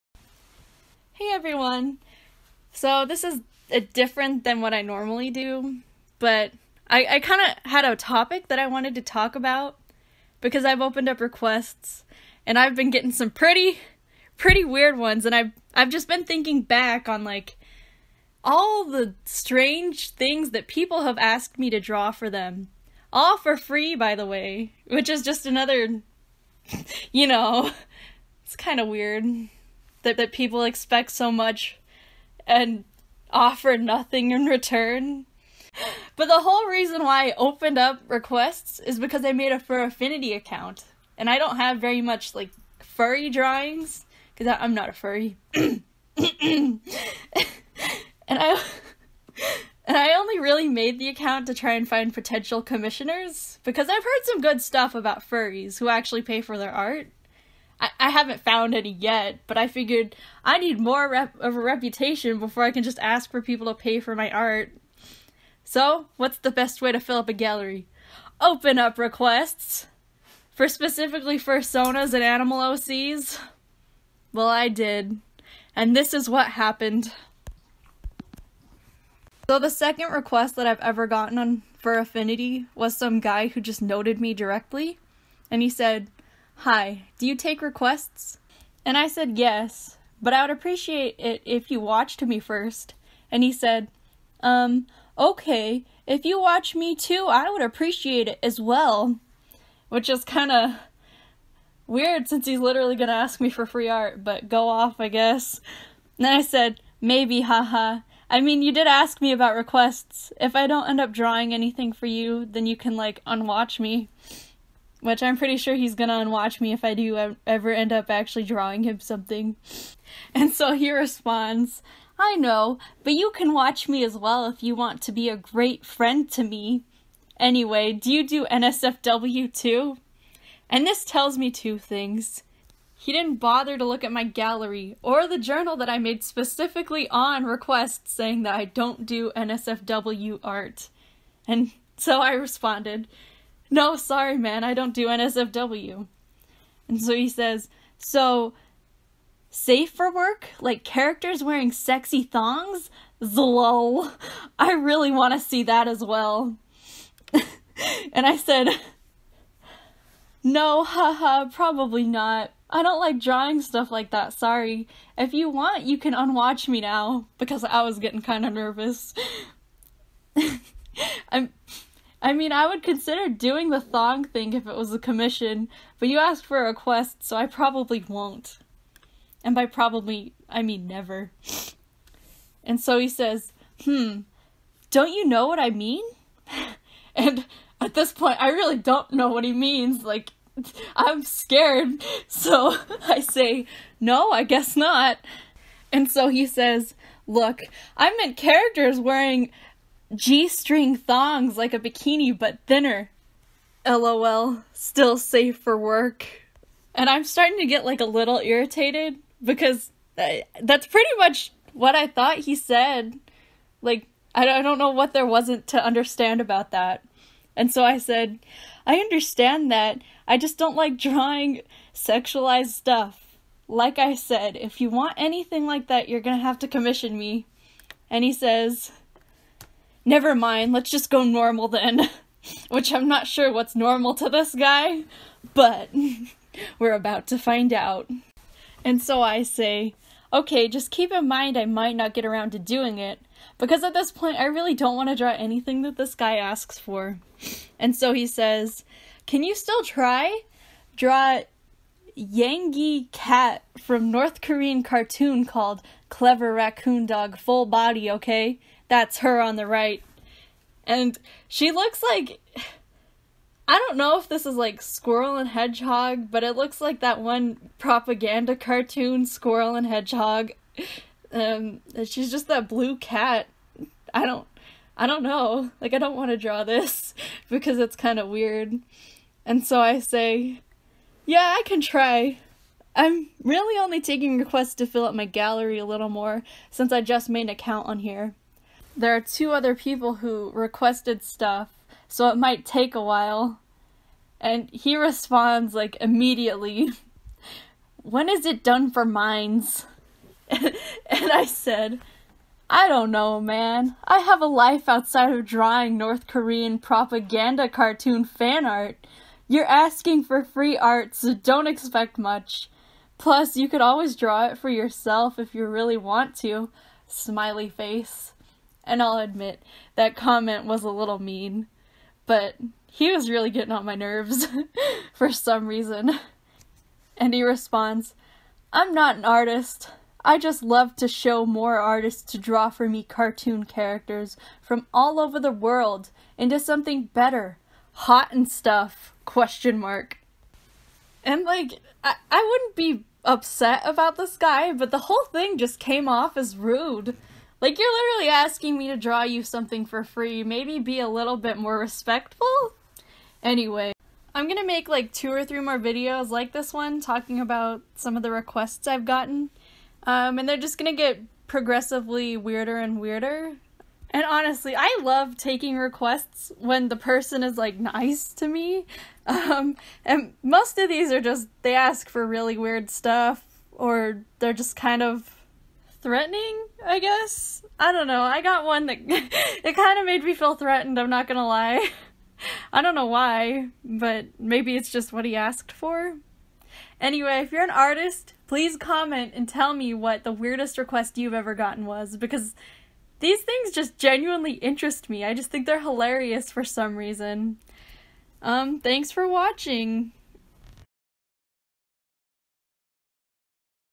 A rant about weird requests (Part 1)
This is my first video like this, so go easy on me

So this is just something that I needed to get out of my system. Maybe people who watch this will relate or something? I'm probably over reacting but requests can be pretty weird sometimes.

If you can't draw and you ask someone else to draw for you, please be nice about asking and never pressure them into doing something they don't want to.

Also the last video in this series will be about yoshi and diapers so get excited.